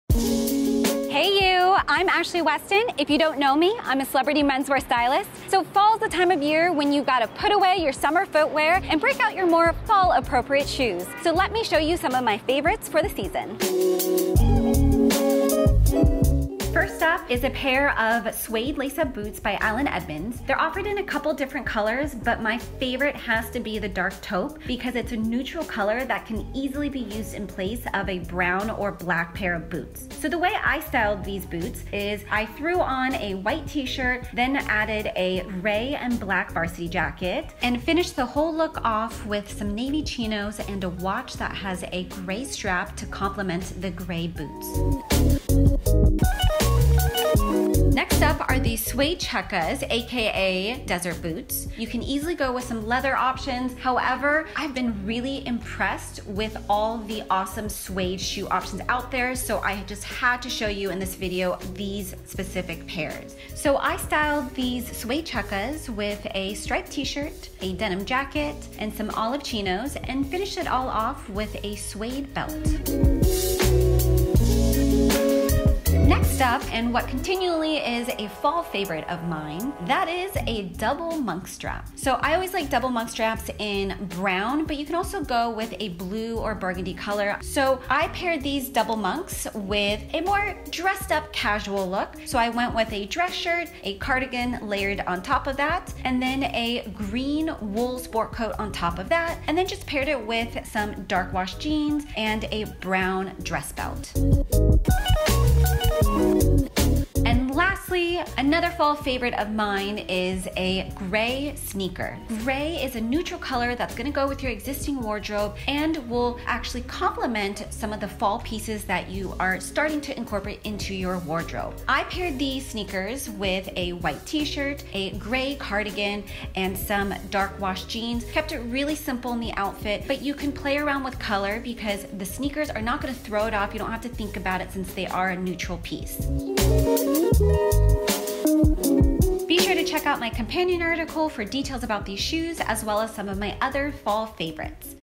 Best Men's Fall Boots & Shoes for 2017
Dress better than ever before with The Essentials App.
In this video, I wanted to show and discuss some of my favorite boots and shoes for the Fall & Winter seasons. See below for links & outfits details.

Don't miss out on my whole series dedicated to Fall & Winter Essentials in the "Related Videos" section below.

FREE MEN'S STYLE EBOOKS

SOCIAL MEDIA & EMAIL NEWSLETTER

RELATED & MENTIONED VIDEOS